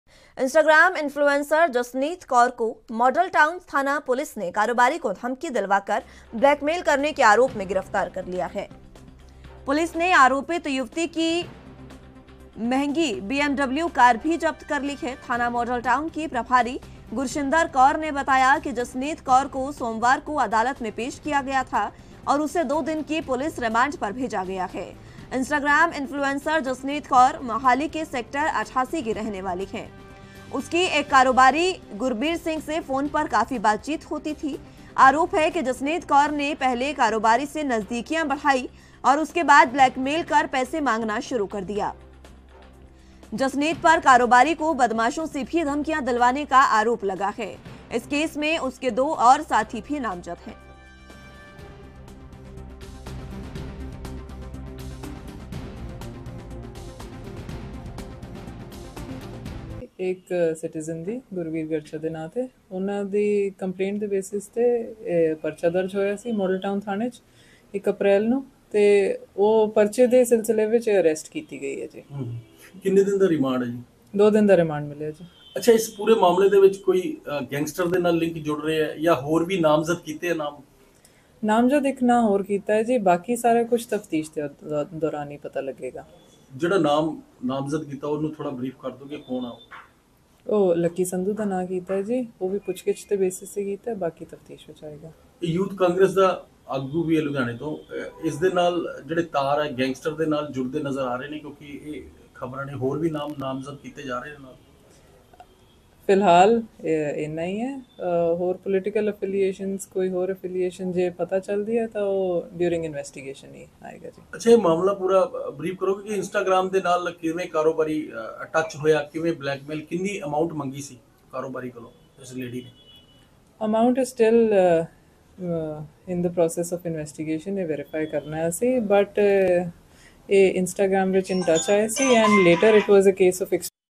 इंस्टाग्राम इन्फ्लुएंसर जसनीत कौर हुईं गिरफ्तार | ANB NEWS
इंस्टाग्राम इन्फ्लुएंसर जसनीत कौर हुईं गिरफ्तार
कारोबारी को ब्लैकमेल करने का लगा आरोप